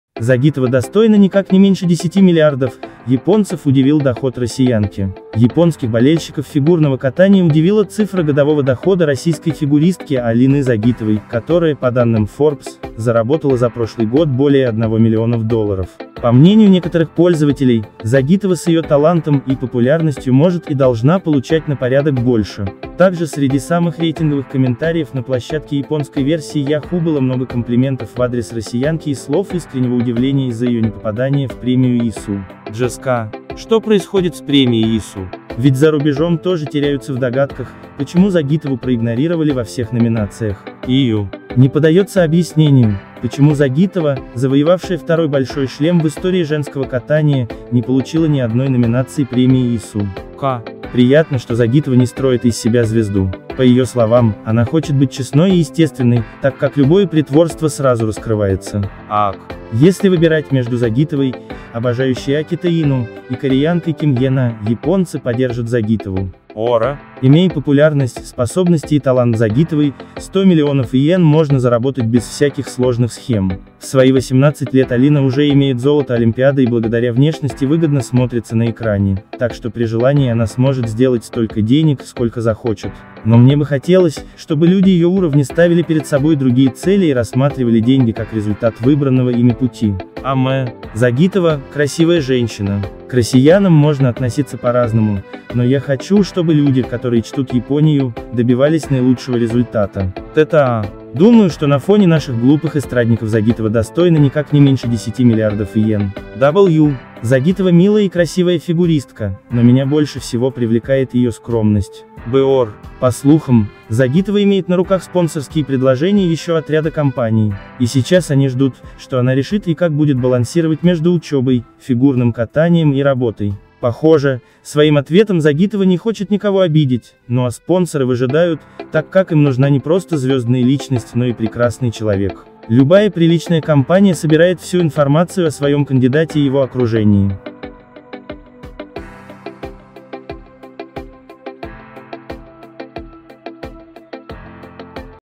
«Загитова достойна никак не меньше 10 миллиардов» - японцев удивил доход россиянки.
Японских болельщиков фигурного катания удивила цифра годового дохода российской фигуристки Алины Загитовой, которая, по данным Forbes, заработала за прошлый год более 1 миллионов  долларов . По мнению некоторых пользователей, Загитова с ее талантом и популярностью может и должна получать на порядок больше.

Также среди самых рейтинговых комментариев на площадке японской версии Yahoo было много комплиментов в адрес россиянки и слов искреннего удивления из-за ее непопадания в премию ISU.

Jks:
- Что происходит с премией ISU? Ведь за рубежом тоже теряются в догадках, почему Загитову проигнорировали во всех номинациях.

Yui:
- Не поддается объяснению, почему Загитова, завоевавшая второй Большой шлем в истории женского катания, не получила ни одной номинации премии ISU.

Ka:
- Приятно, что Загитова не строит из себя звезду. По ее словам, она хочет быть честной и естественной, так как любое притворство сразу раскрывается.

Ukc:
- Если выбирать между Загитовой, обожающей акита-ину, и кореянкой Ким Ен А, японцы поддержат Загитову.

Ora:
- Имея популярность, способности и талант Загитовой, 100 млн иен можно заработать без всяких сложных схем. В свои 18 лет Алина уже имеет золото Олимпиады и благодаря внешности выгодно смотрится на экране, так что при желании она сможет сделать столько денег, сколько захочет.
Но мне бы хотелось, чтобы люди ее уровня ставили перед собой другие цели и рассматривали деньги как результат выбранного ими пути.

Ume:
- Загитова – красивая женщина. К россиянам можно относиться по-разному, но я хочу, чтобы люди, которые чтут Японию, добивались наилучшего результата.

Tqd:
- Думаю, что на фоне наших глупых эстрадников Загитова достойна никак не меньше 10 млрд иен.

W:
- Загитова милая и красивая фигуристка, но меня больше всего привлекает ее скромность.